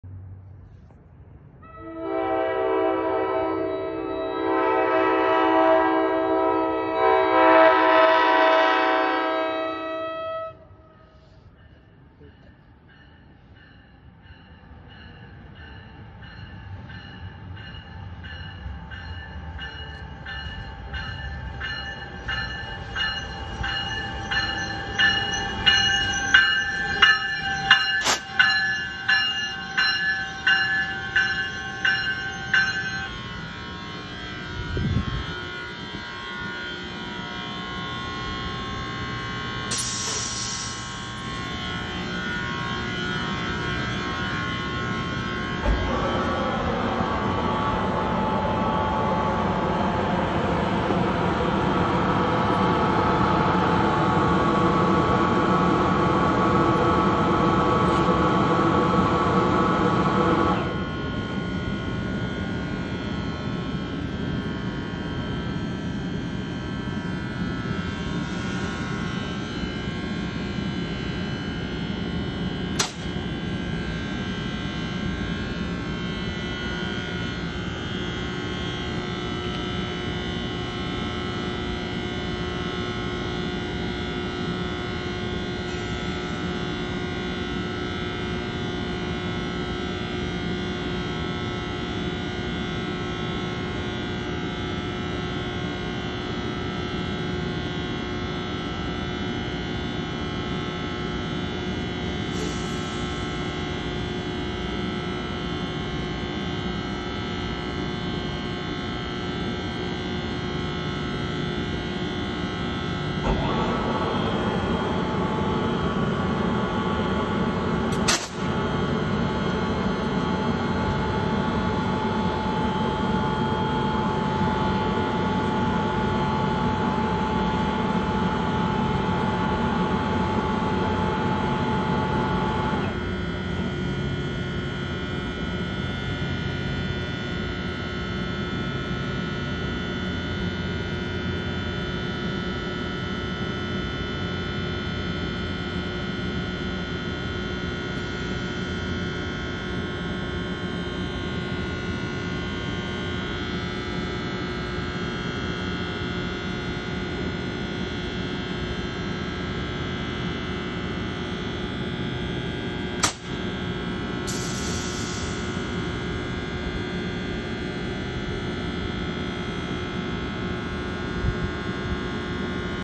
ALP-45DP 4527 on train 877
This so called "hamster locomotive" leads this train to Hackettstown. I'm not a big fan of these engines at all and wishing I would see either a PL42AC, F40PH or GP40PH. I'm not a fan of the electric fan sound in the back of the locomotive. This train took like forever to leave the station, either because of us on the platform to board or something was going on in one of the coaches. Enjoy!